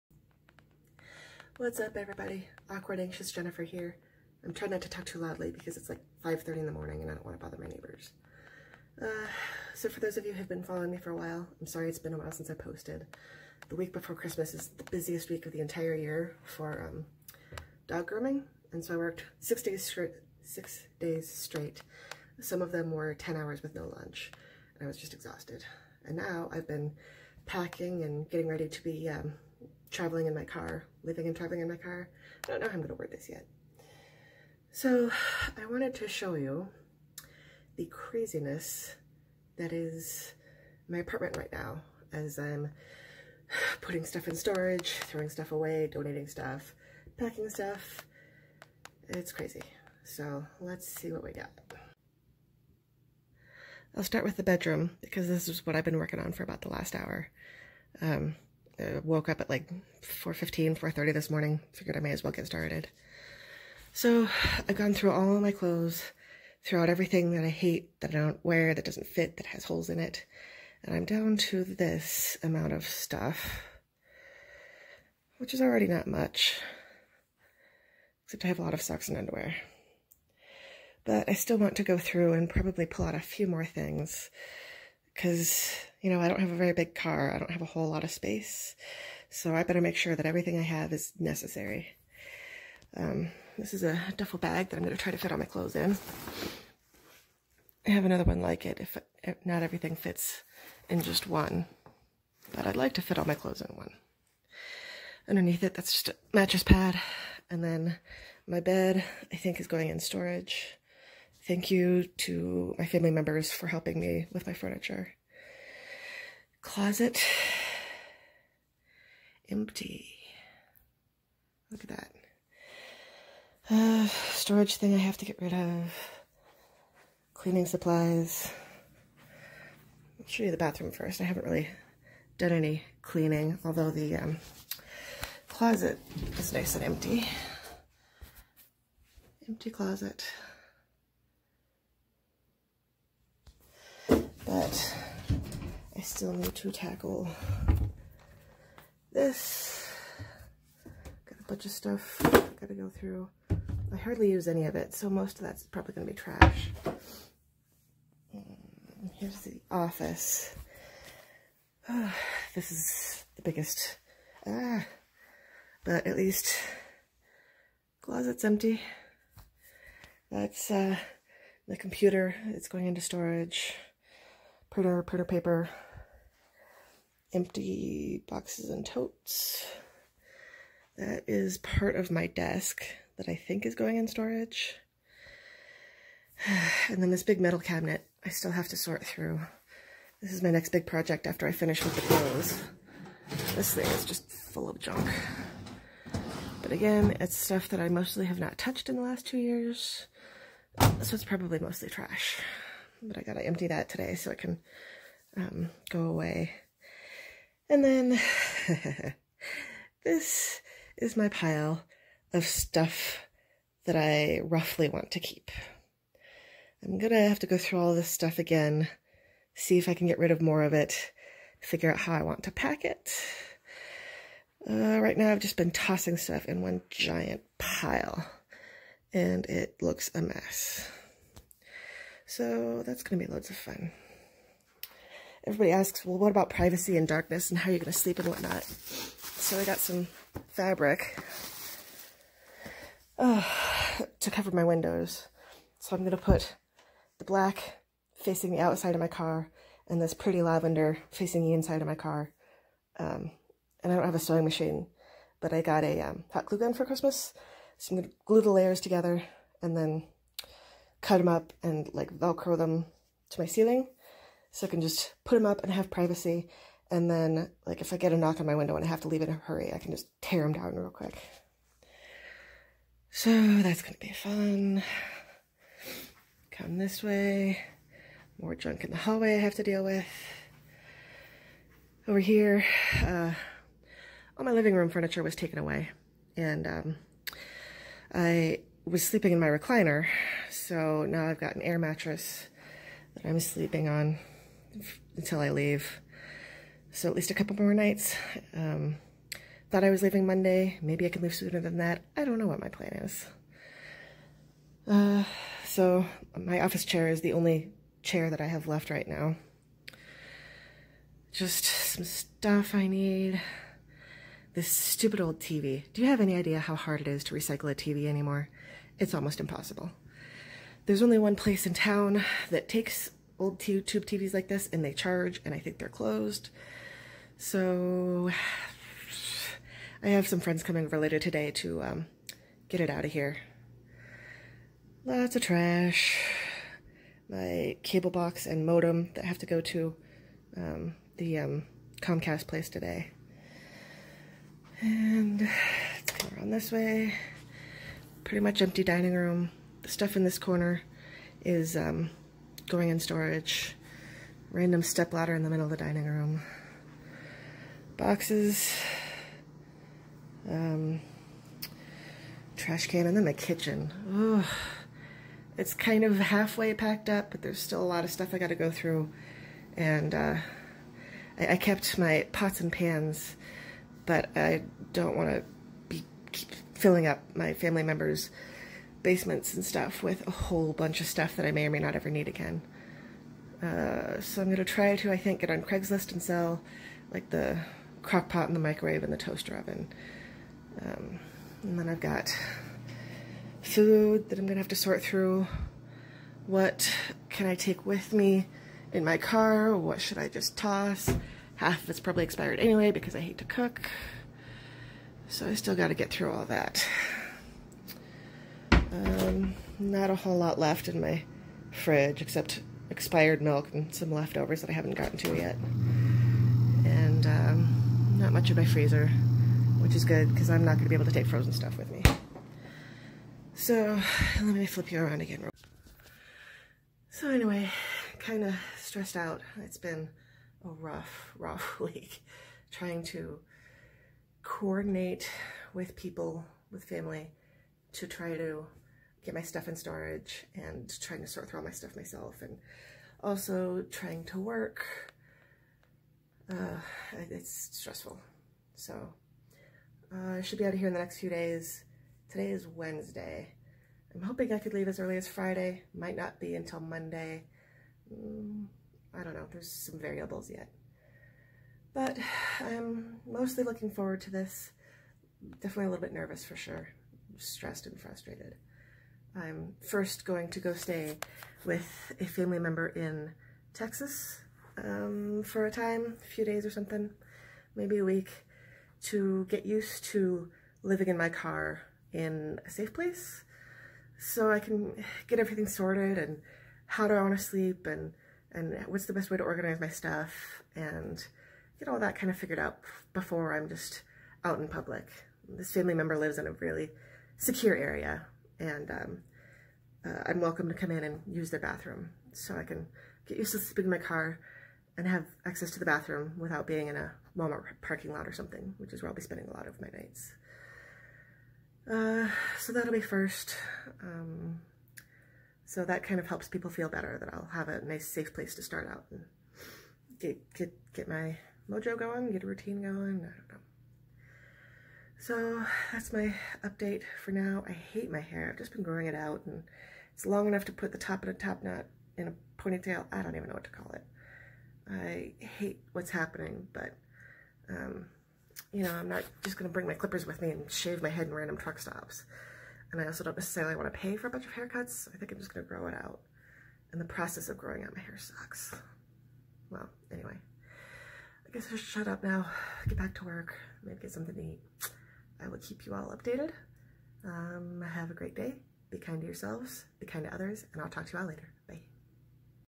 Getting ready to be living in a car
I'll be living in a car pretty soon so I can travel the country, but getting everything out of a 2-bedroom apartment is super stressful!